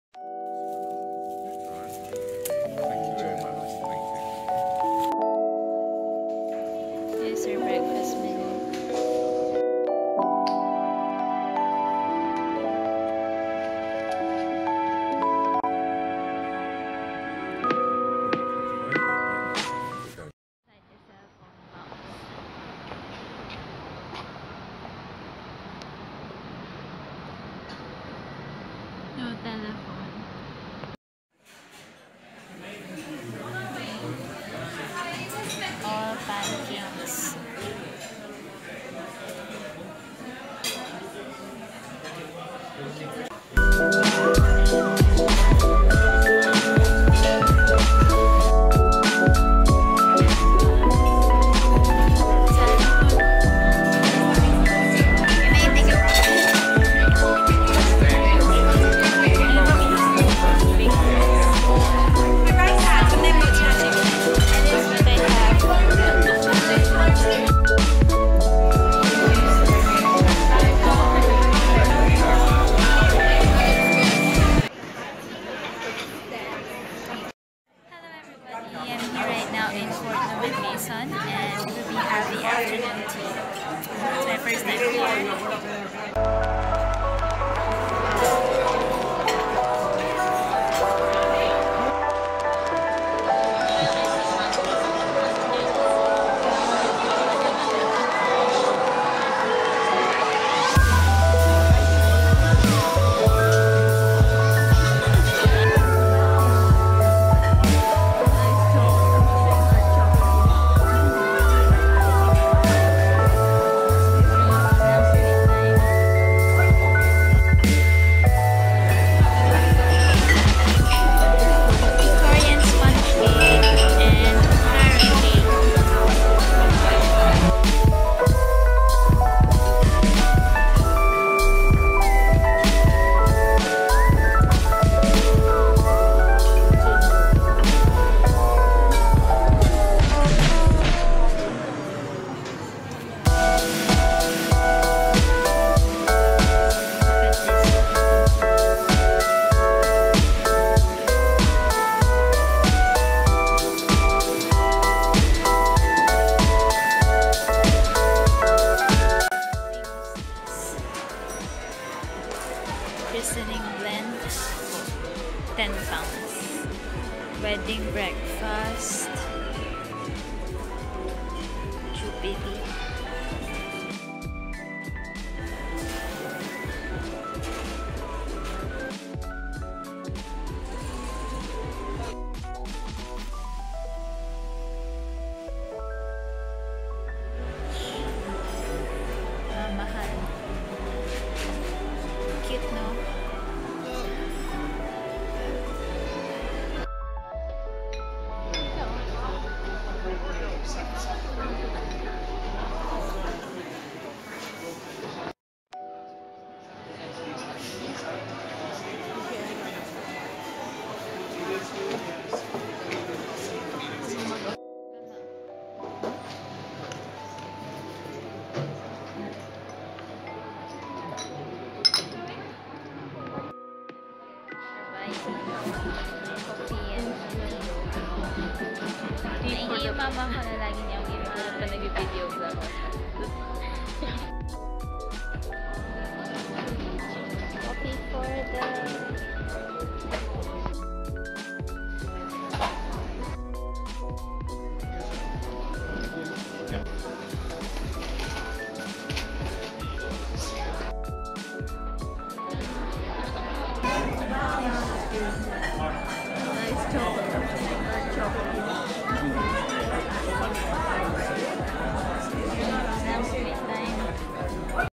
VLOG #4: PANCAKE OVERLOAD + FORTNUM AND MASON LONDON
Early celebration of the National Pancake Day in London! I didn't know we were heading to a pancake place for lunch so I ordered Pancakes with sausage in McDonald's. For lunch, we went to My Old Dutch in Holborn, which is famous for their pancakes with a twist. We also had afternoon tea in Fortnum & Mason.